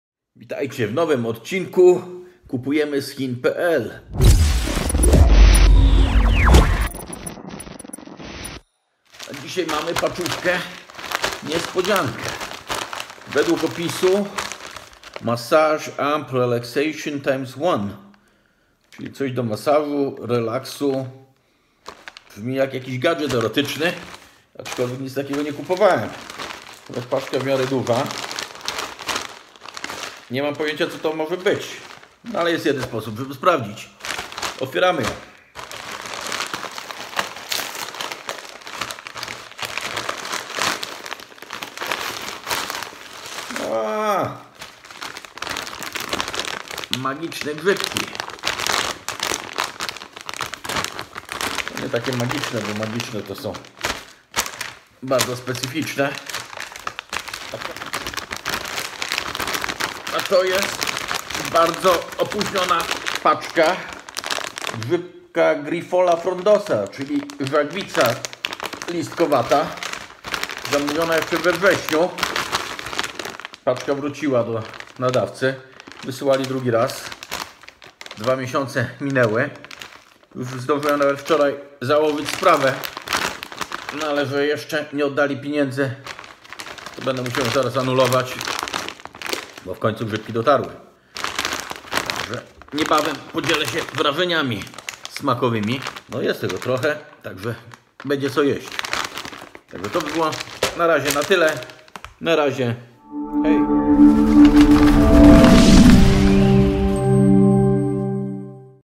Grifola Frondosa - Żagwica Listkowata
Czy znacie jakiegoś innego grzyba, który ma tyle nazw? Oprócz powyższych, jest jeszcze Maitake, huba siedź, huba gałęzista, żagiew gałęzista, żagiew krzaczasta, żagiew cykoriokształtna, wielogłówka listkowata.
Na szczęście w końcu dotarł, aczkolwiek Chińczyk u którego kupiłem tą wielką pakę poniżej $10, niestety już wycofał z oferty i teraz widzę tylko na eBay’u tutaj w paczkach po 100g, 200g i 500g, ale już za wyższą cenę.